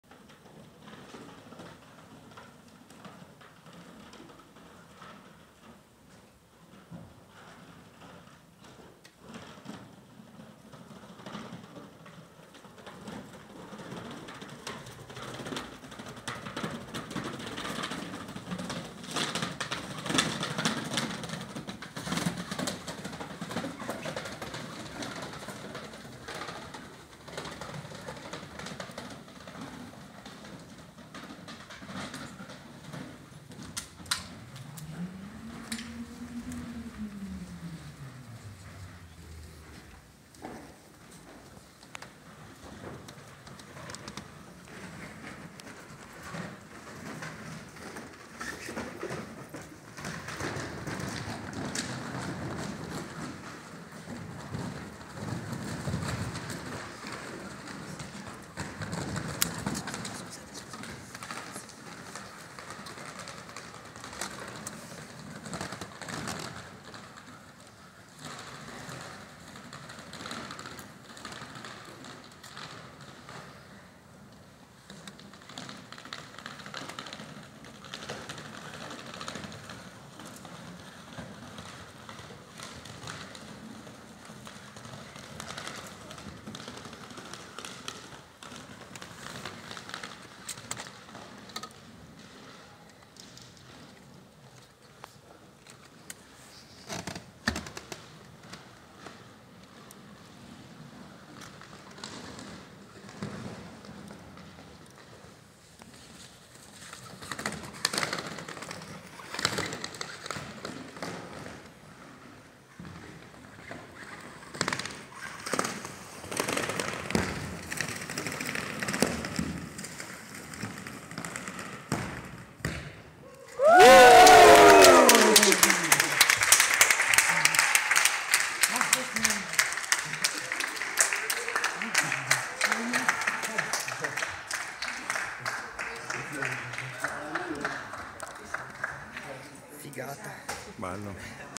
Record mondiale di domino di libri - Bücherdomino: Weltrekord
Nel campus di Bolzano, è stato infranto il record mondiale di domino di libri: 6.631 i volumi utilizzati per costruire il percorso snodatosi attraverso i quattro piani della biblioteca. Il precedente record mondiale, di 5.337 libri, era stato stabilito nel 2014 dalla biblioteca dell’Università di San Gallo in Svizzera.
An der Freien Universität Bozen wurde ein neuer Rekord im Bücherdomino gebrochen: 6.631 Bücher fielen über die Stockwerke der Universitätsbibliothek und haben damit den bisherigen Weltrekord von 5.337 Bücher getoppt. Letzterer wurde von der Universitätsbibliothek St. Gallen gehalten.